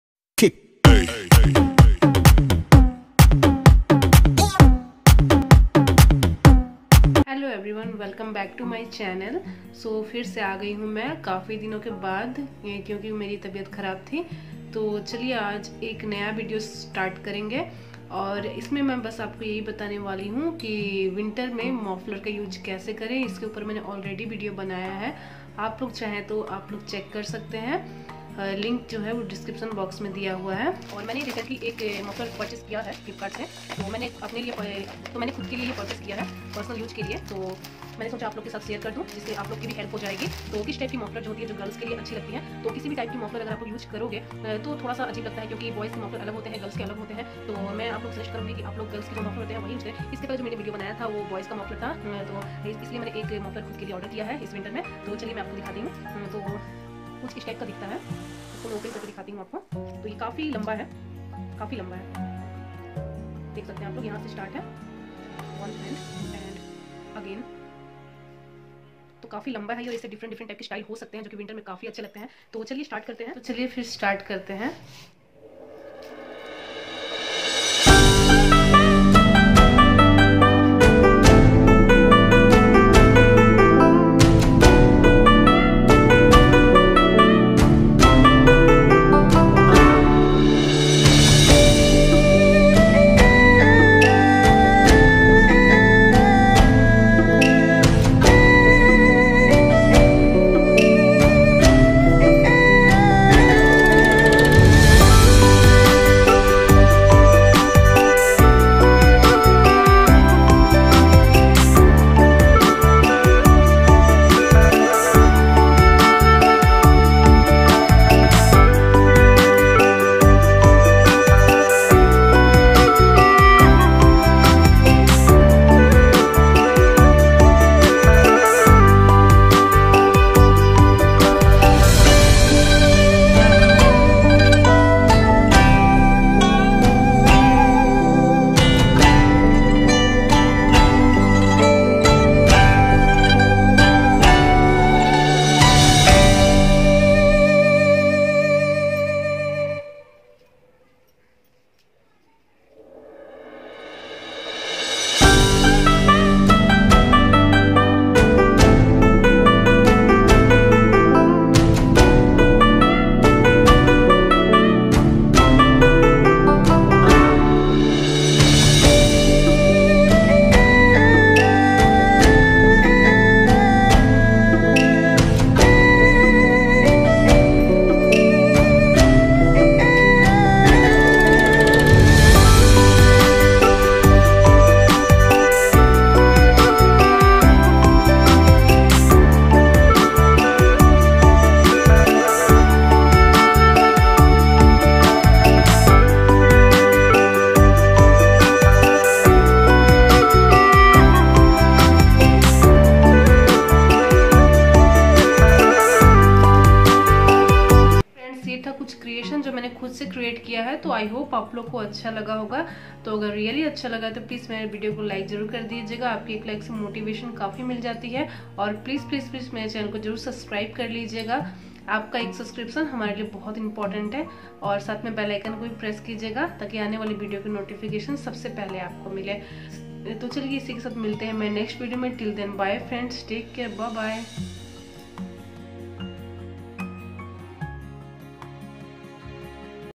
9 Ways to Wear a Scarf // Muffler // Super Stylish Ideas // Indian Youtuber Rina
Hi,

Friends this is the video is about How to style a Scarf or muffler in winter in 9 different  ways. This Idea will be very helpful for every Girls or women.